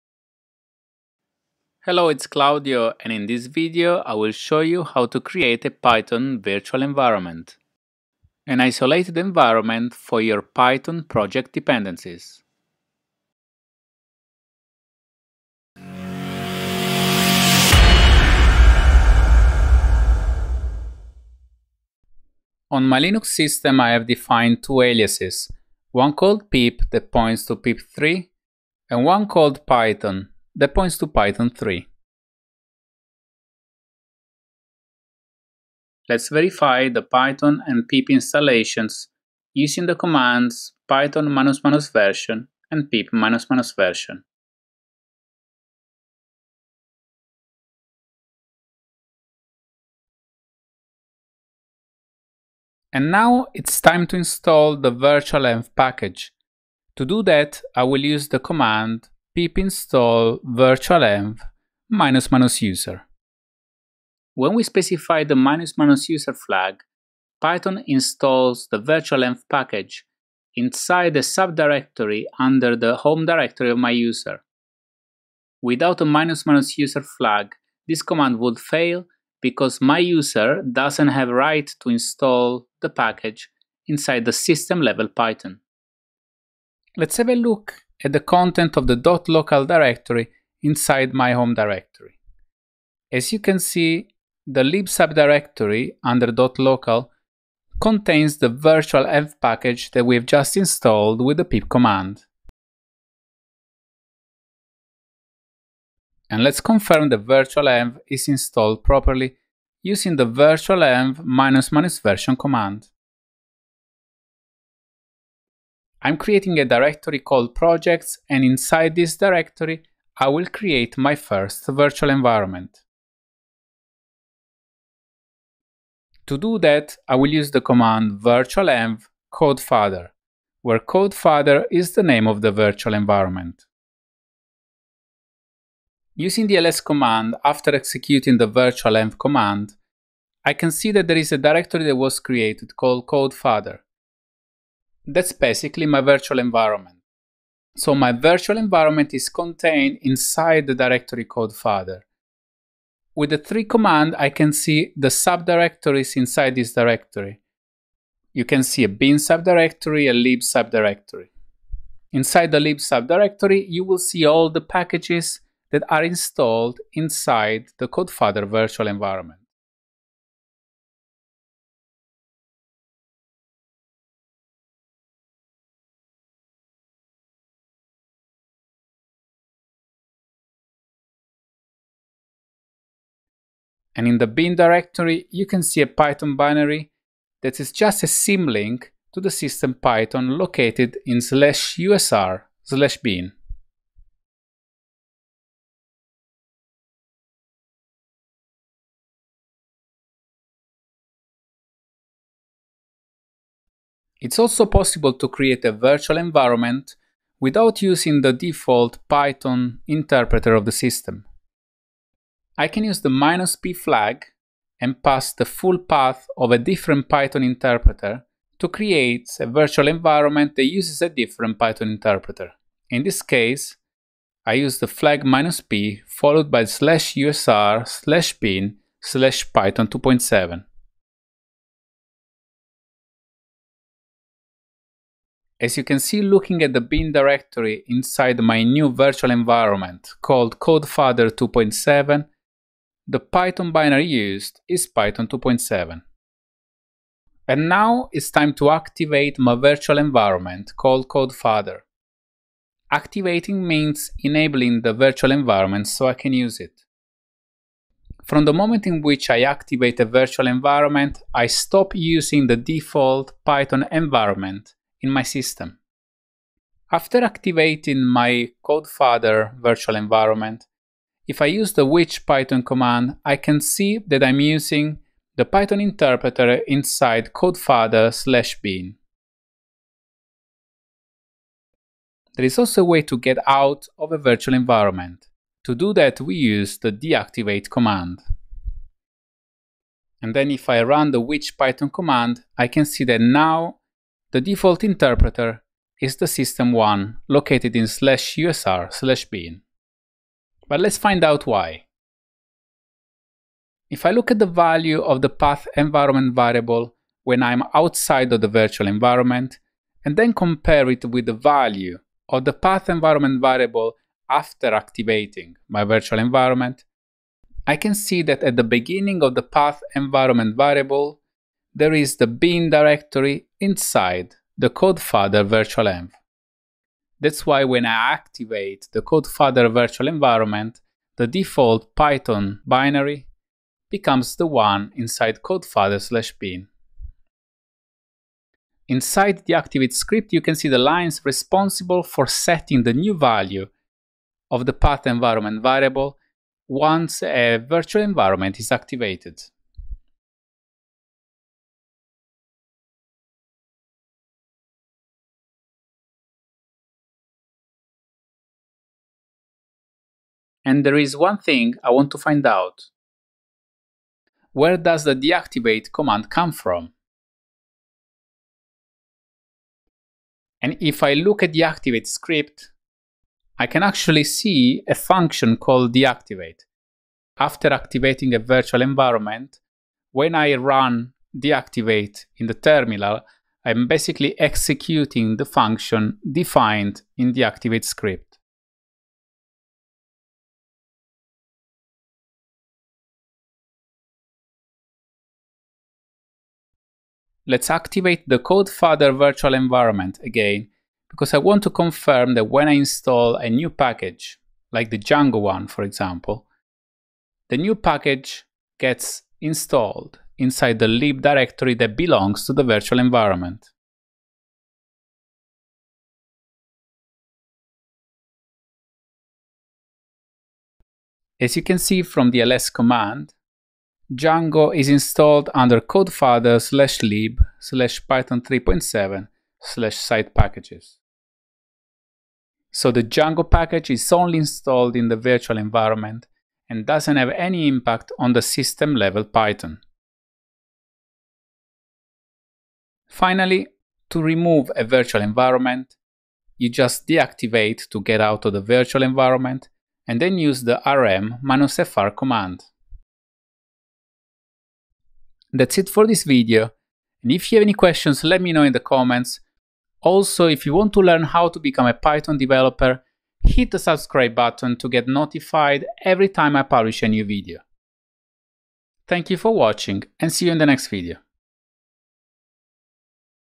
Set Up Your Python Virtual Environments on Linux: A Quick Guide
This CodeFatherTech video shows how to create a Python virtual environment on a Linux system.

Python virtualenv allows you to create an isolated environment where to keep all your project dependencies without interfering with the global Python environment.

In this video, you will learn how to create a virtual environment, how to understand its structure, how to activate and deactivate it, and how to delete it.